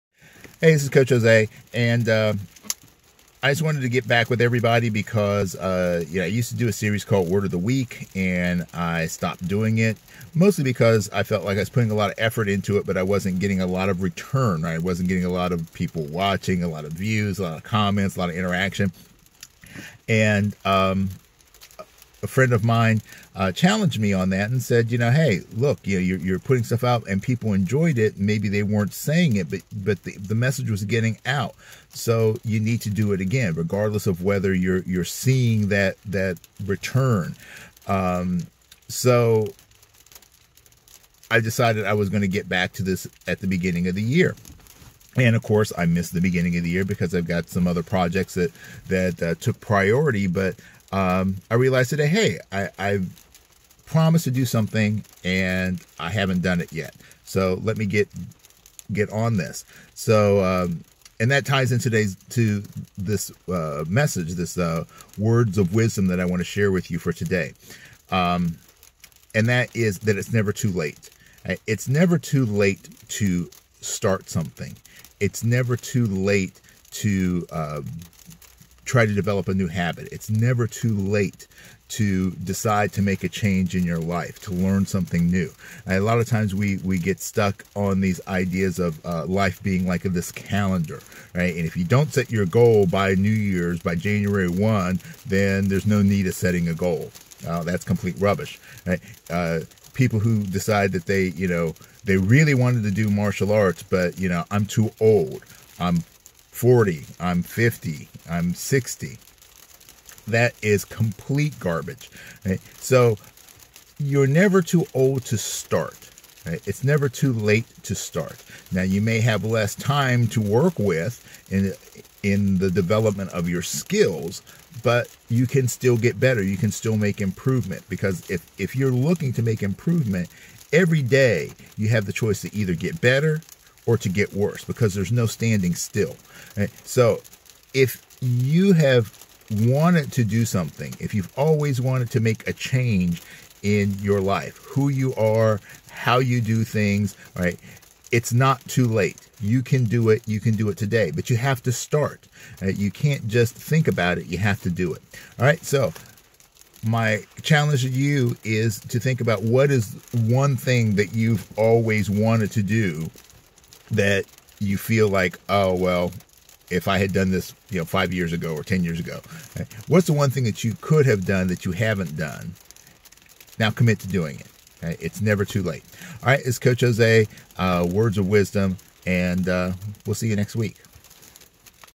Words of Wisdom Episode #1: "It's Never Too Late"
Have you ever caught yourself saying something like "I wish I had done this when I had the chance" or "I'm too old to start"? Well you are not! It's never too late.